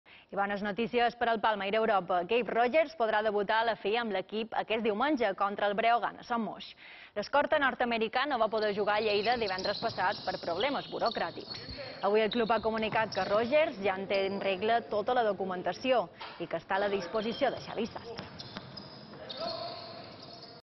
Gabe Rogers podrà debutar diumenge
Amb una setmana de retard,  Gabe Rogers ja està a disposició de Xavi Sastre pel partit de diumenge. Una vegada solucionats els seus problemes burocràtics, l'escorta debutarà al Palau de Son Moix contra el Breogán.